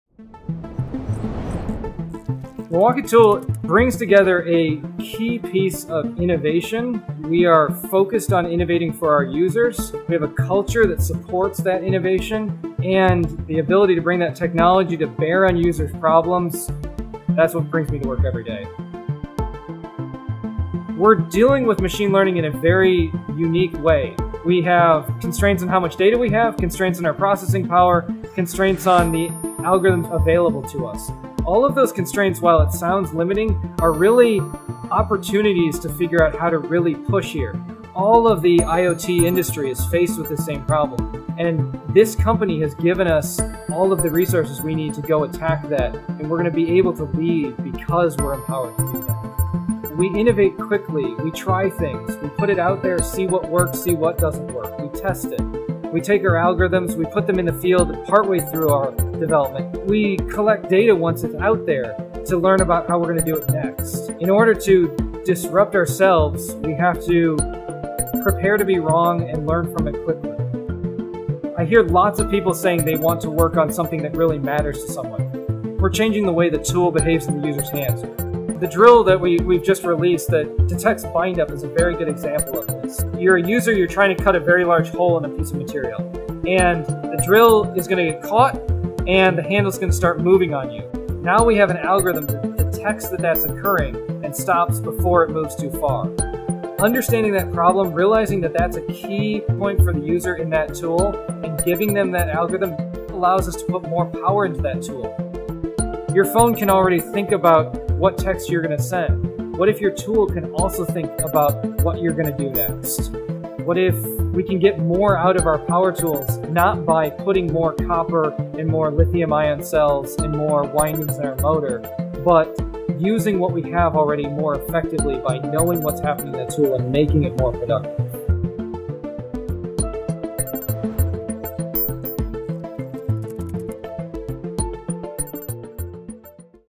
MILWAUKEE® Pioneering the Next Revolution Machine Learning in Power Tools
Milwaukee Tool's team discusses their focus on innovation and how they use machine learning to improve their products. Despite constraints in data, processing power, and algorithms, Milwaukee Tool sees these challenges as opportunities to push themselves further. They test their algorithms in the field and quickly learn from any mistakes. Their goal is to create tools that matter to their users, like their recent drill that can detect bind up and prevent accidents. The team believes that with machine learning, they can make power tools more productive without adding more materials to them.